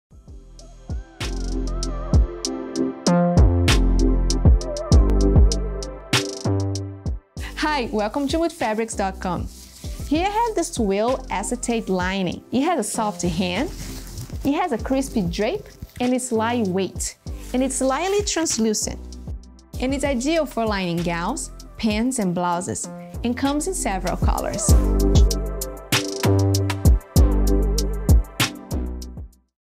Mood Fabrics Granada Twill Acetate Lining
Make an entrance boasting with confidence when wearing a garment complemented by this Granada White Twill Acetate Lining. Take a walk on the smooth, yet breathable side of things when wearing pieces that brush gently against the skin, providing an airy touch. Don’t forget to appreciate the properties presented by acetate linings such as static resistance and little to no shrinking. You won’t find stretch or full opacity in this lightweight textile, but a top layer or overlay will pair nicely to provide more coverage and structure. A lustrous sheen radiates off the surface of this fabric for a luxurious appeal. Unveil unparalleled garments when using this fluid material to line billowing ball gowns, sheer georgette blouses, pleated chiffon pants, and so much more!